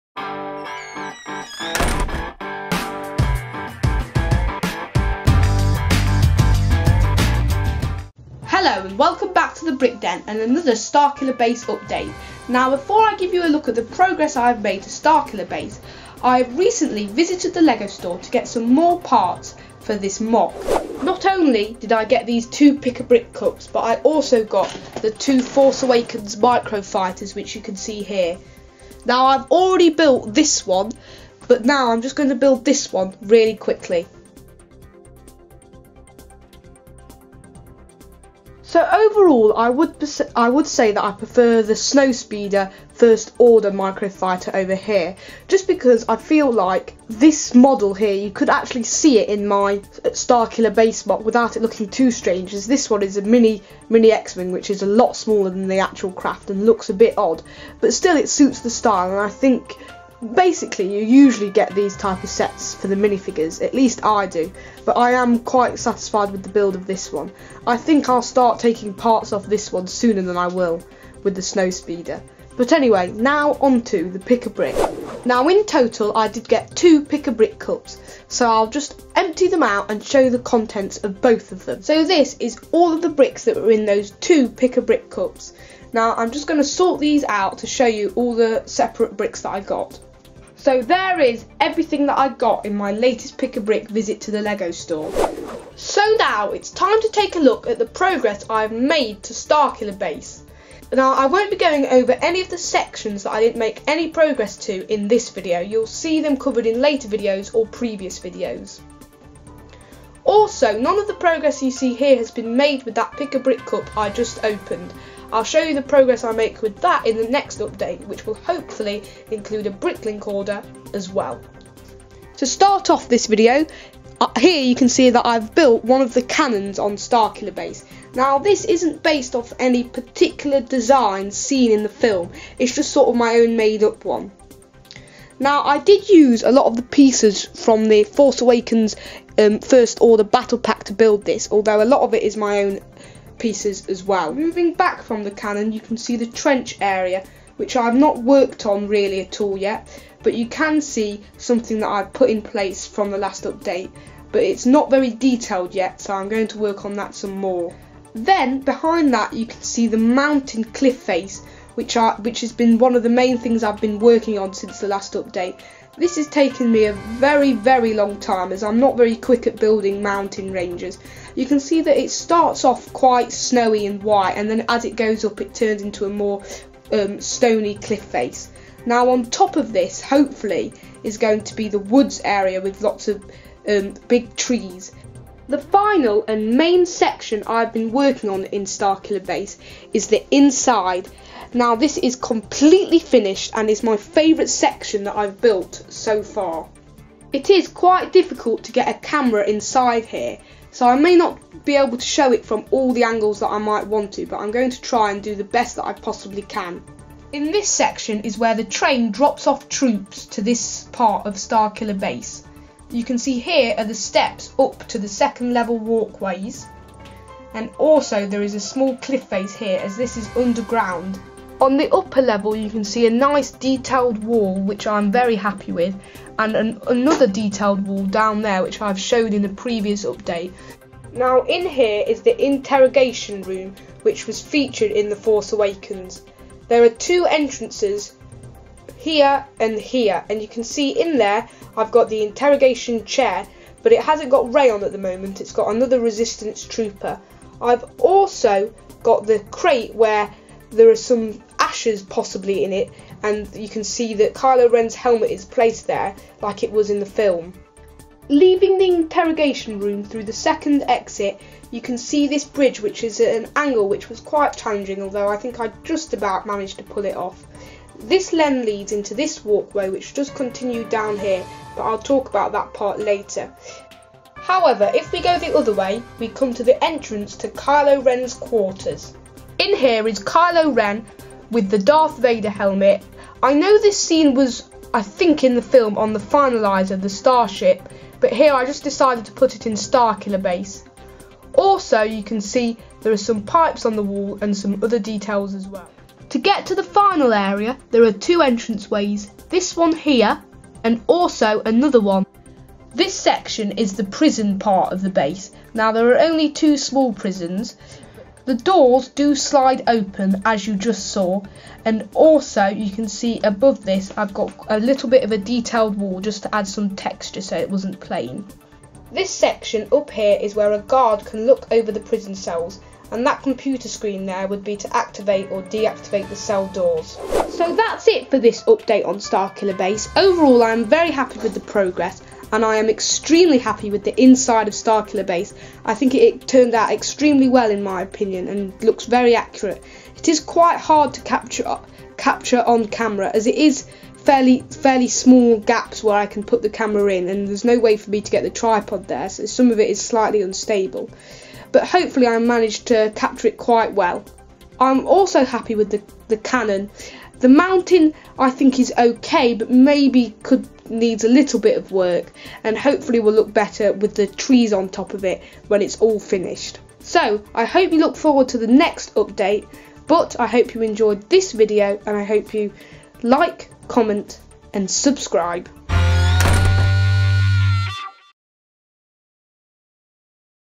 Custom Lego Starkiller Base: Update 3-Mountain/Turret/Interior (Lego Force Awakens M.O.C)
In this video I show you the progress I have made to Starkiller Base. Also I open two Pick-A-Brick cups from my latest visit to the Lego store. The bricks will be used for Starkiller Base and you will see the progress I make with them in the next update. Finally I also give my thoughts on the two Force Awakens Micro Fighters, which I also got at the lego store!